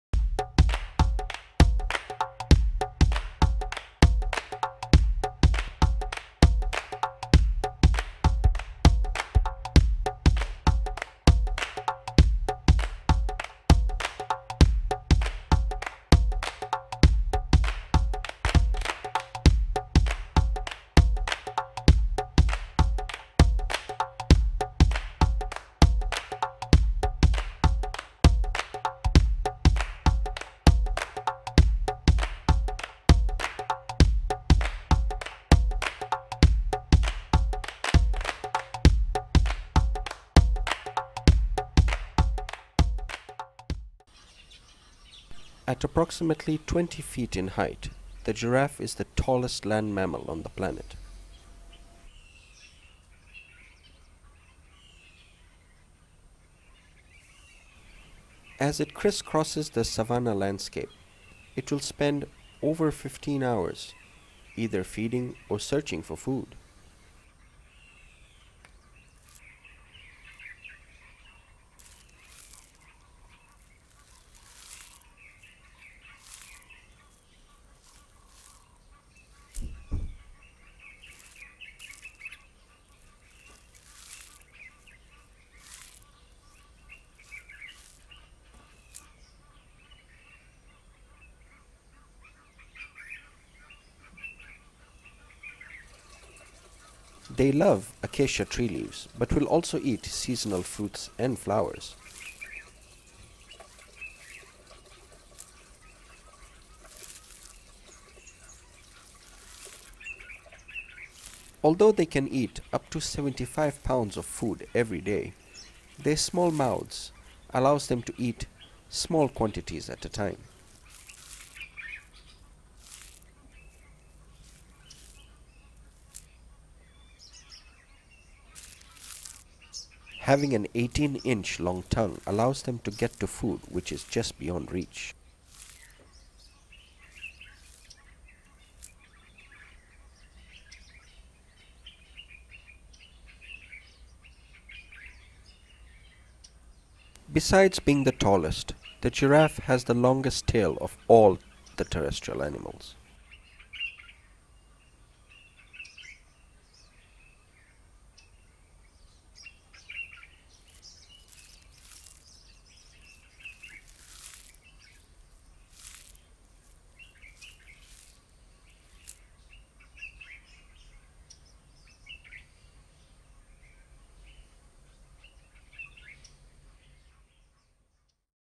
Tsavo Giraffe Eating Small Fruits and Acacia Leaves
An adult male Masai Giraffe walks towards a tree in Tsavo East National Park. It stretches its long neck and tongue to begin feeding on leaves and small fruits in the tree canopy.

Filmed By & Narrated by:
Shazaad Kasmani